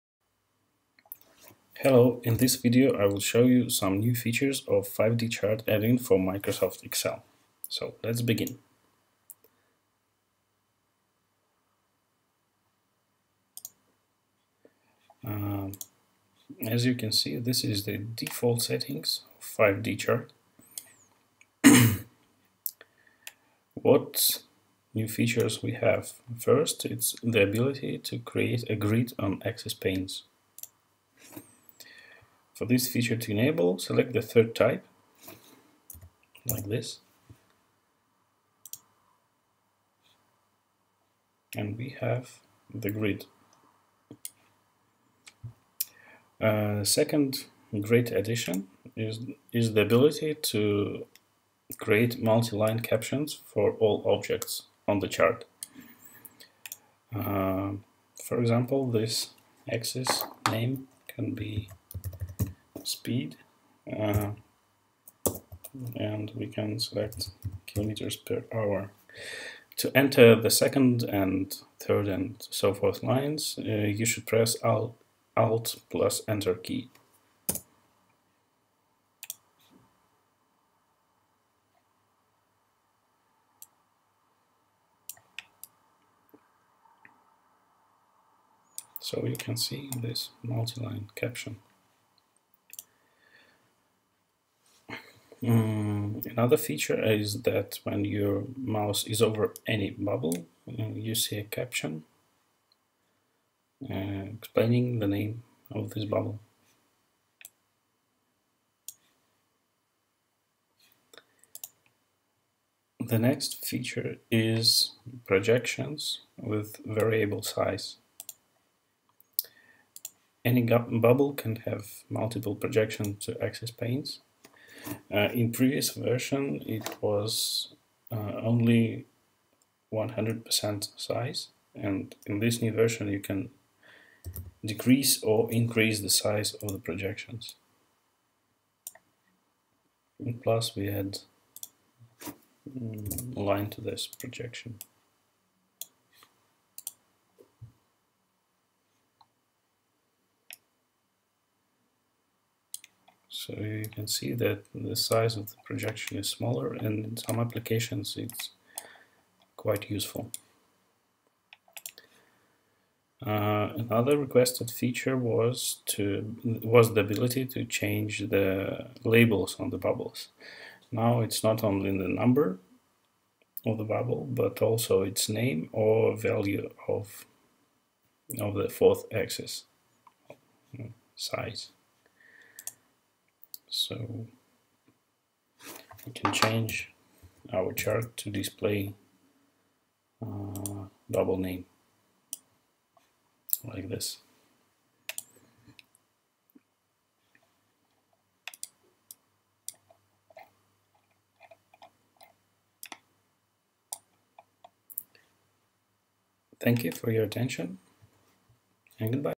5dchart Add-In
5dchart Add-In for MS Excel

Real 3D bubble chart and 3D scatter plot directly in MS Excel! Profesional easy-to-use tool for multi-dimensional data visualization.